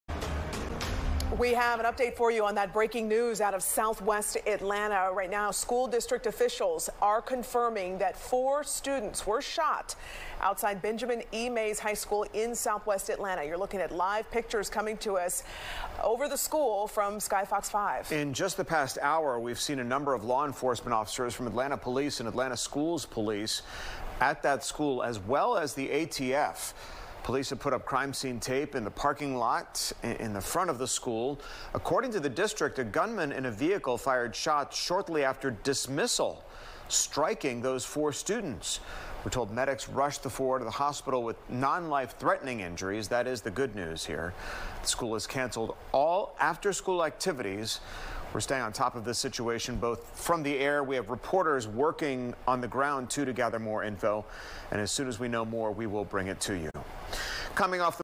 Mays High School students shot on campus | FOX 5 News
Four Benjamin E. Mays High School students were shot in the campus parking lot.